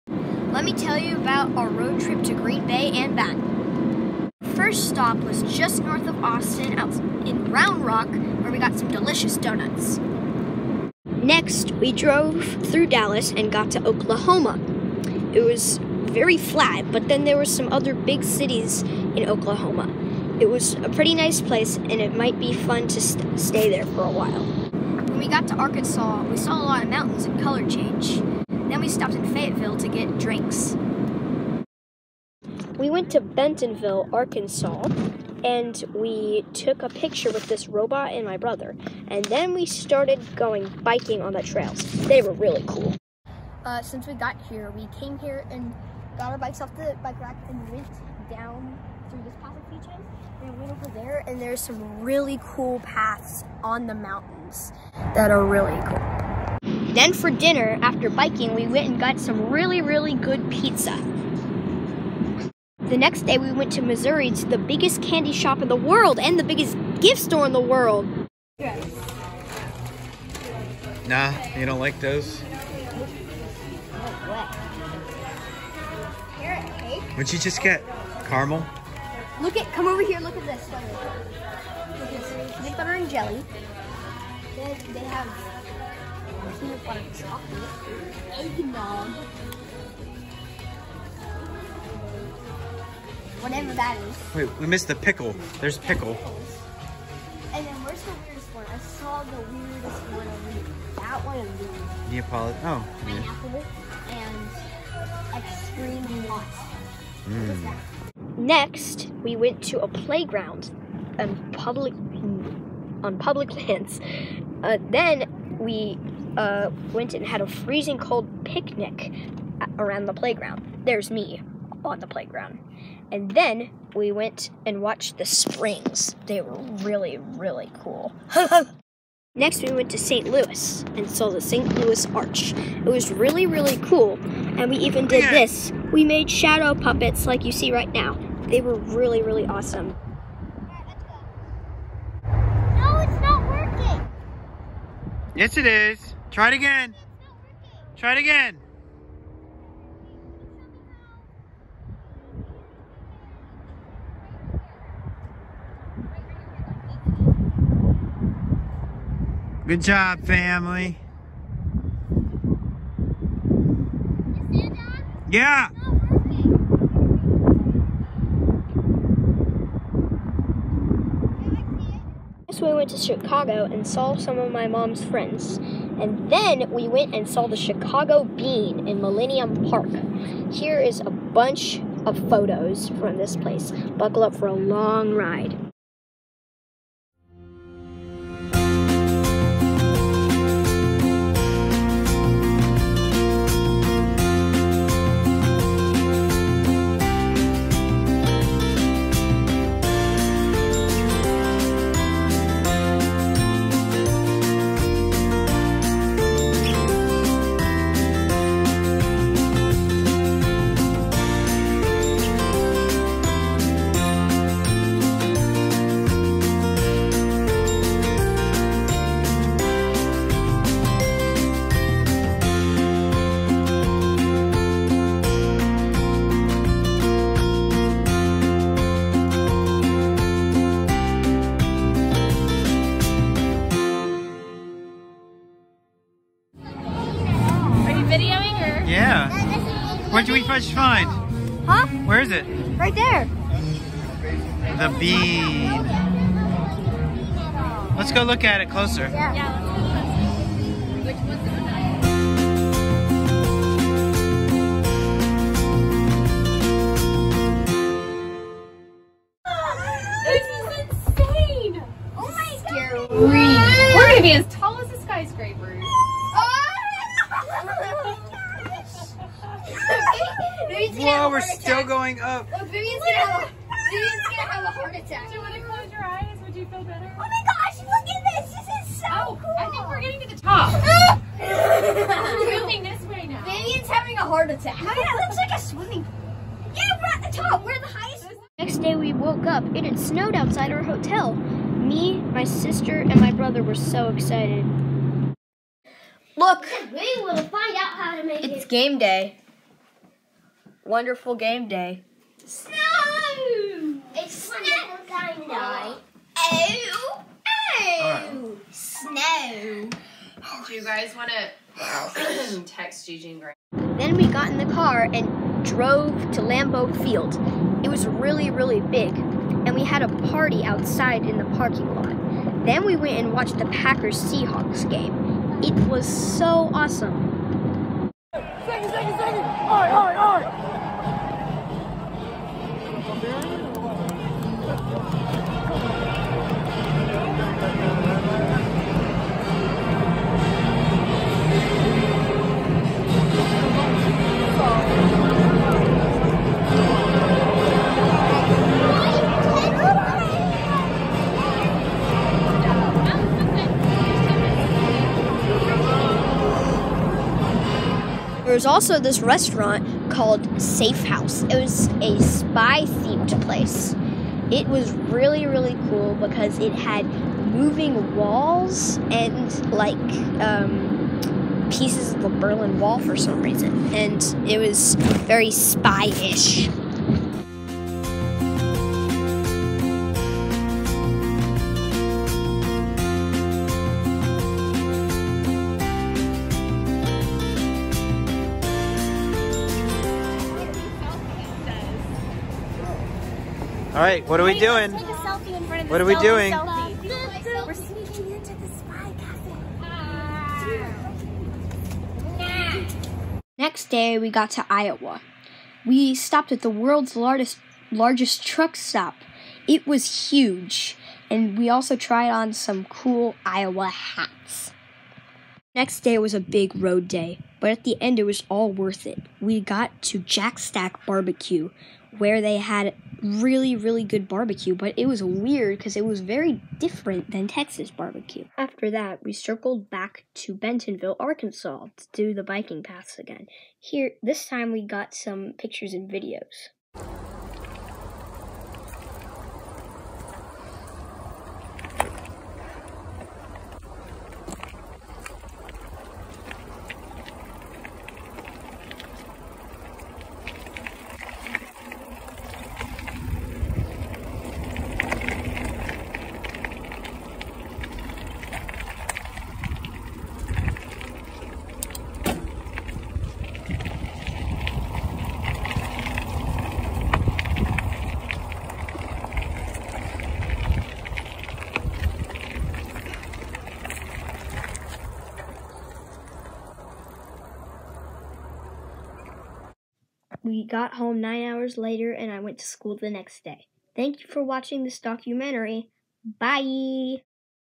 Road trip! Kellers Texas to Green Bay
This is a summary of my Road trip to Green Bay and back.